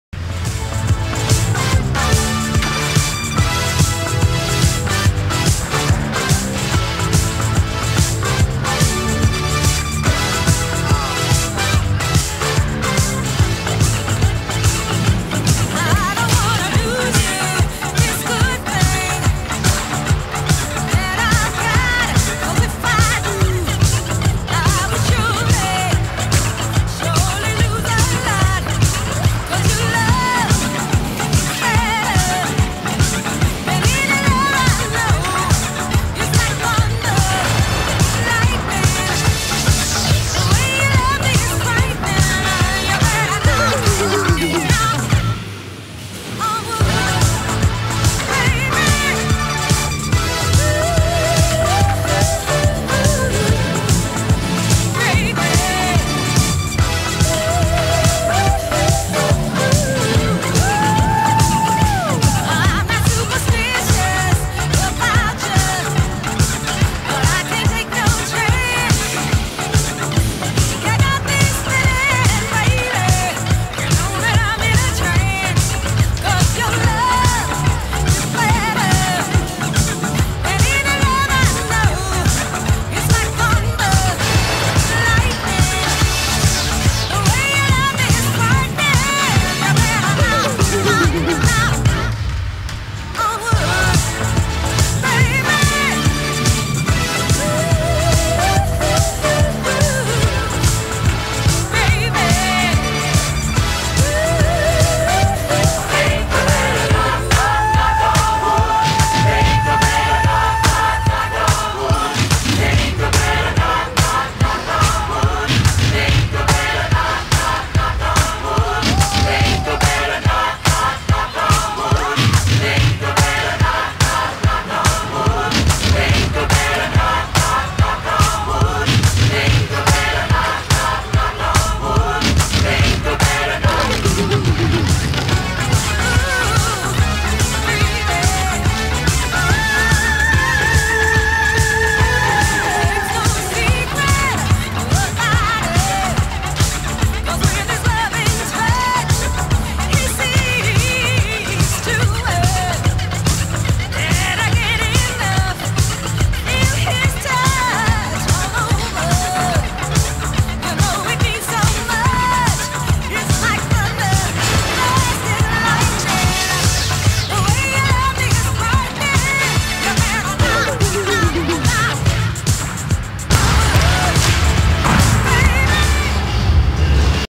SouthParcSession5Janvier2008
Filmed By:Etienne Dumais,Ismael Kouyaté,Charles.
William Cristofaro Jean Sebastien Lapierre Alex Doing bACK tO bACKS And Will Skating South January 5th